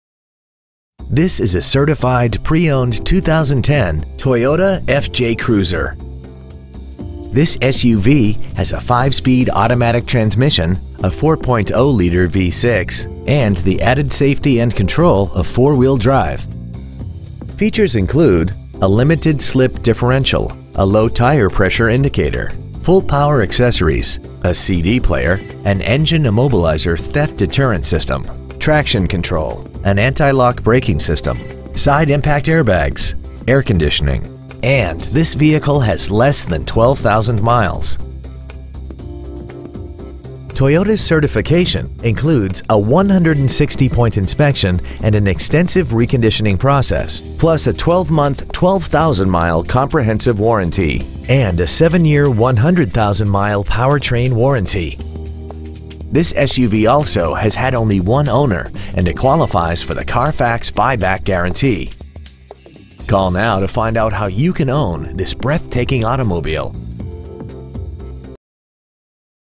2010 Toyota FJ Cruiser Houston Texas 77034
Year : 2010
 Make : Toyota
 Model : FJ Cruiser
 Engine : Gas V6 4.0L/241
 Trans . : Automatic
 Exterior : Black
 Miles : 11,010
 Interior : Gray